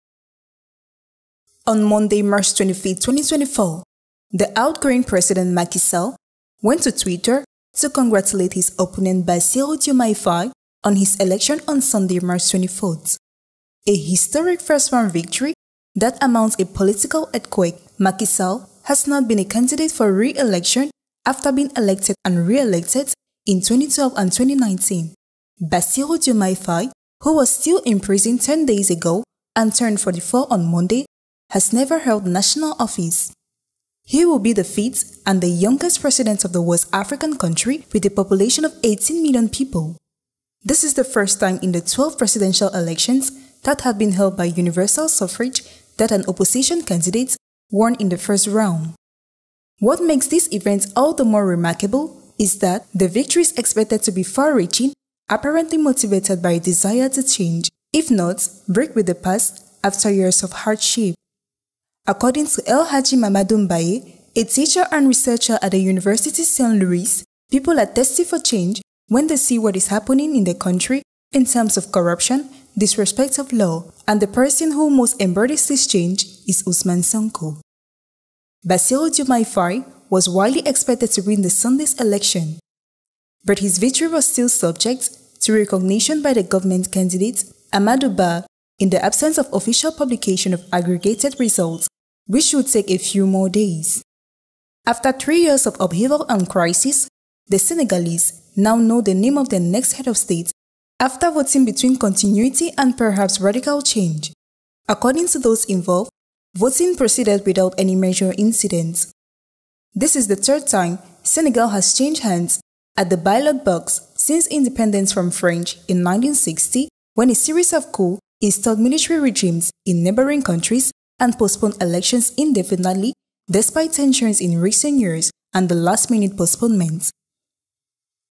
SENEGAL ELECTION: MACKY SALL "CONGRATULATES" WINNER DIOMAYE FAYE, HIS SUCCESSOR
On Monday 25 March 2024, the outgoing president, Macky Sall, went to X to congratulate his opponent, Bassirou Diomaye Faye, on his election on Sunday March 24th . A historic first-round victory that amounts to a political earthquake.  Macky Sall has not been a candidate for re-election after being elected and re-elected in 2012 and 2019.